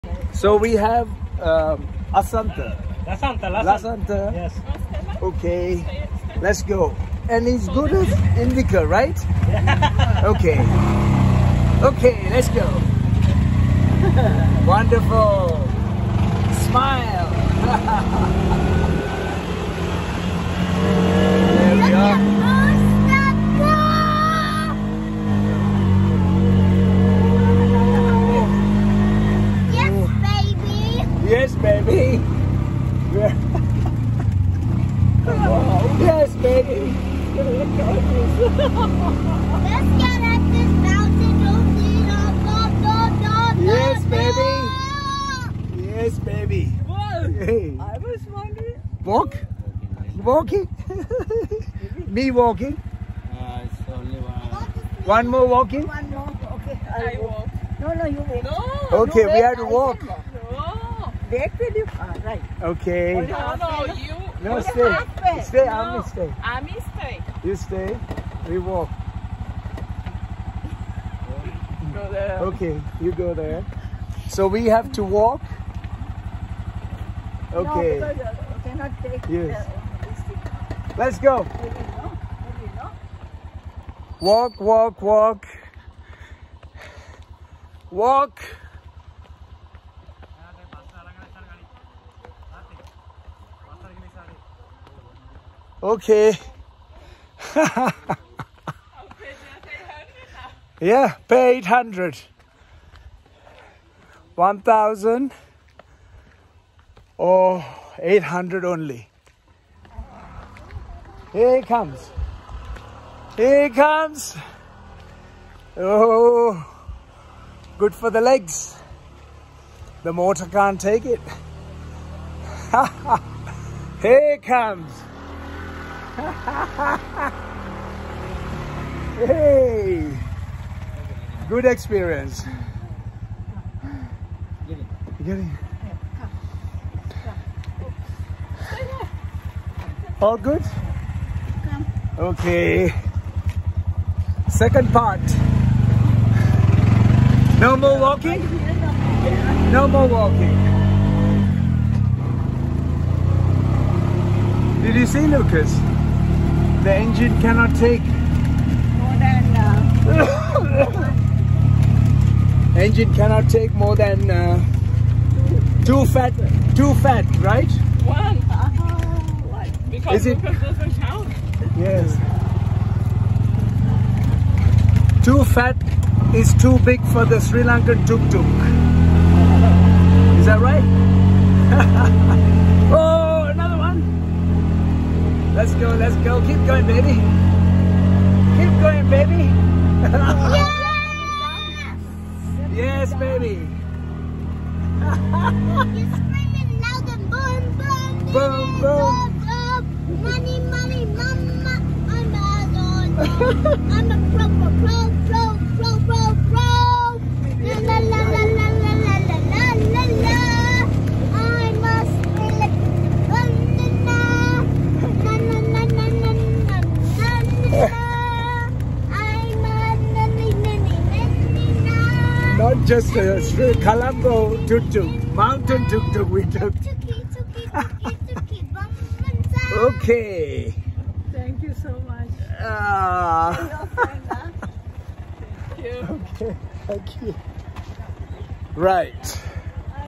Tuk Tuk Advenutre steep hill Ella Sri Lanka
We hired a Tuk Tuk because of time factors to get down and get up to see the nice arches in Ella.

See what happened when the Tuk Tuk had to take my mum and dad and my grandma up the steep hill.